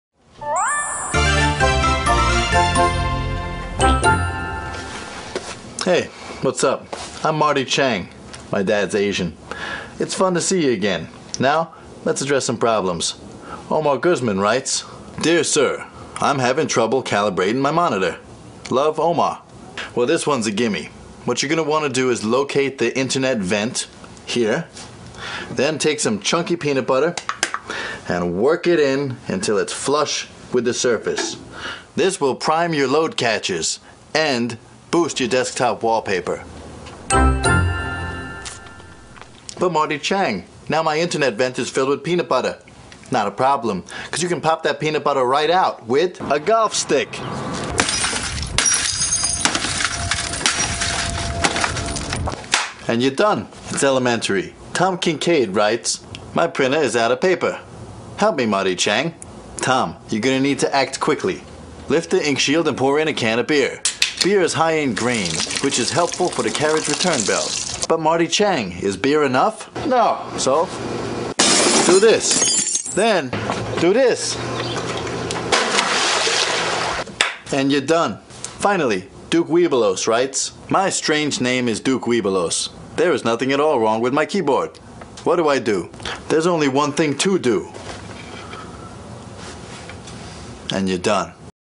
Marty Chang's Computer Repair tips
Can't seem to find the original any longer, just trying to preserve some internet nostalgia from the man who taught me the basics of PC maintenance - Marty Chang.